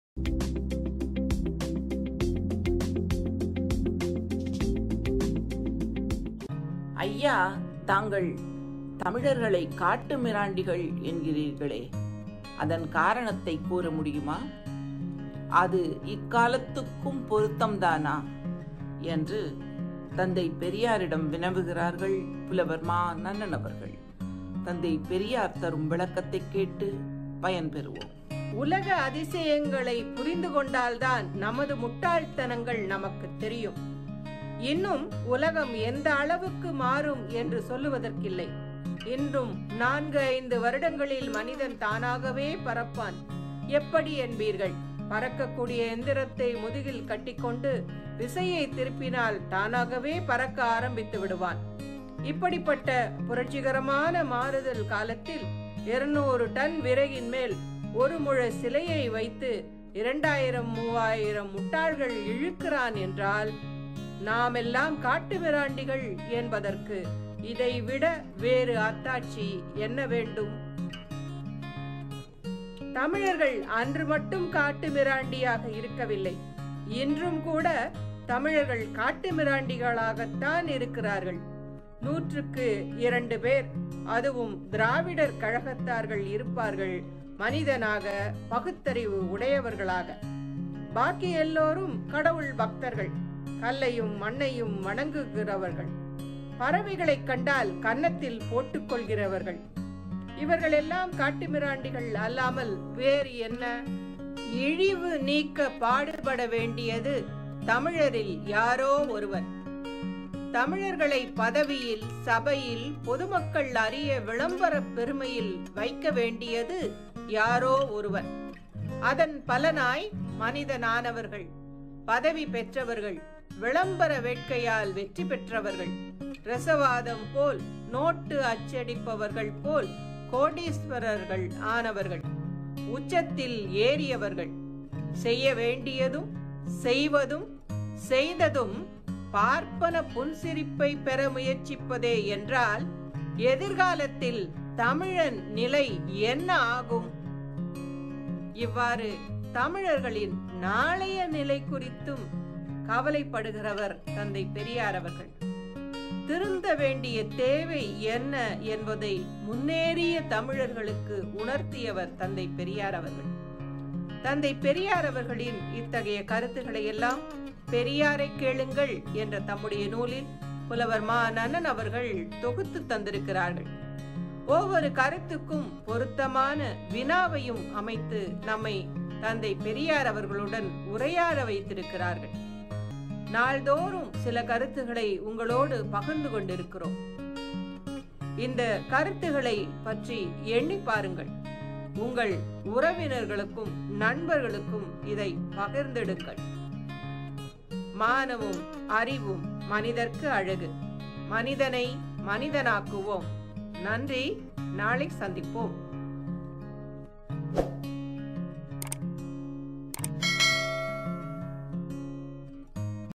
பெரியாரைக் கேளுங்கள் 314
தமிழர்கள் காட்டுமிராண்டிகள் என்று தந்தை பெரியார் அவர்கள் கூறுவதற்கான காராணம் என்ன? / புலவர் மா. நன்னன்